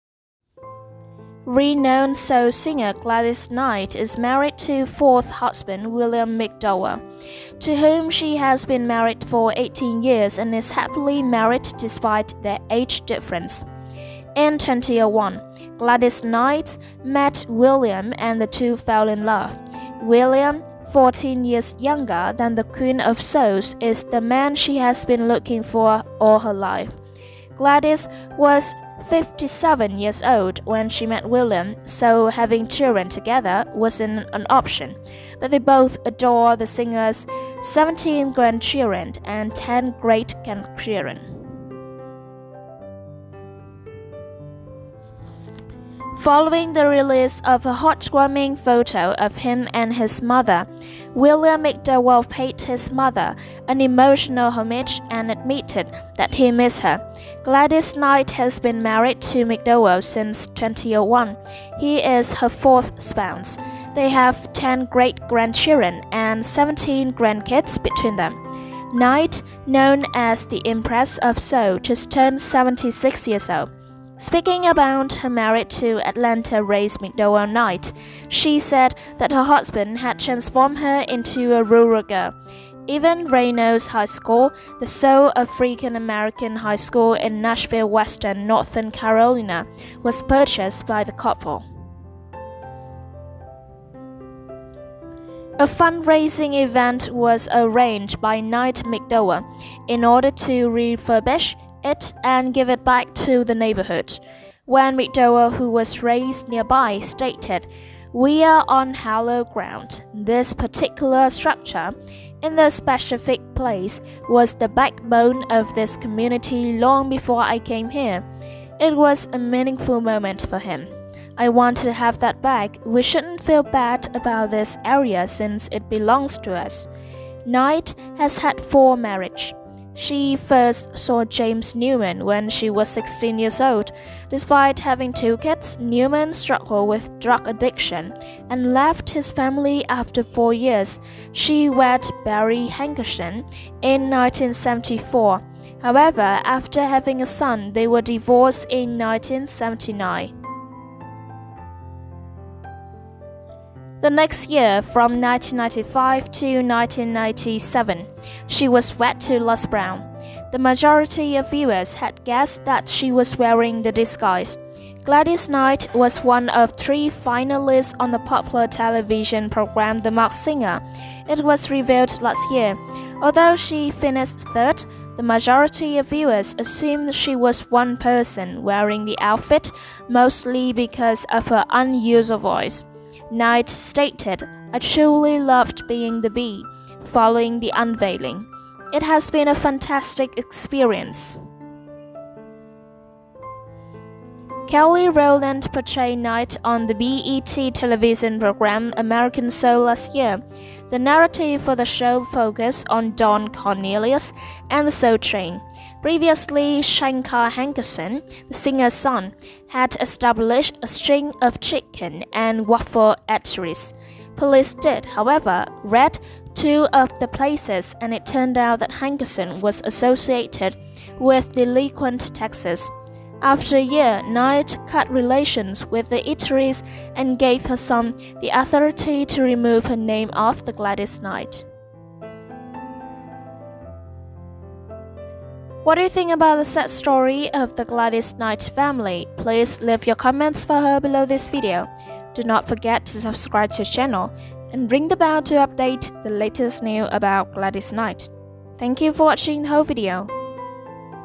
R.I.P. We're Extremely Sad To Share Gladys Knight Is In Mourning And Share The Most Painful Details
Gladys Maria Knight (born May 28, 1944), known as the "Empress of Soul", is an American singer, actress and businesswoman. A seven-time Grammy Award-winner, Knight recorded hits through the 1960s, 1970s, and 1980s with her family group Gladys Knight & the Pips, which included her brother Merald "Bubba" Knight and cousins William Guest and Edward Patten.